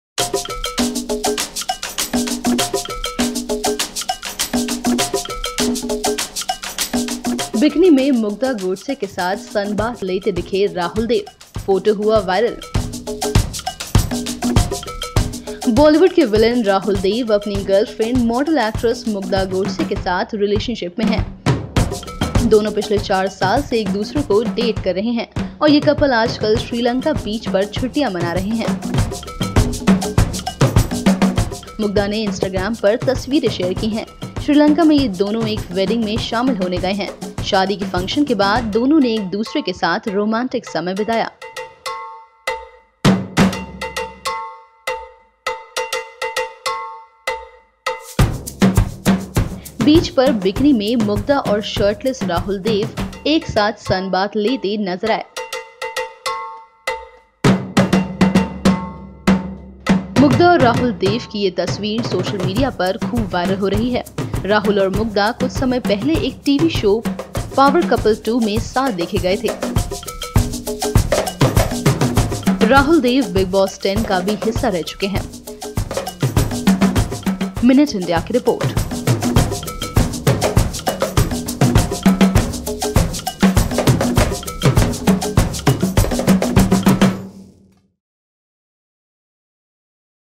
Rahul Dev के साथ कुछ इस अंदाज में Sunbath लेती दिखी Mugdha Godse
Actress Mugdha Godse, who is currently in Sri Lanka with boyfriend Rahul Dev, seems to be enjoying her vacation a lot in the foreign land. She is sharing adorable and fun pictures from her holiday diaries with Rahul Dev on Instagram, giving her fans and followers major relationship goals.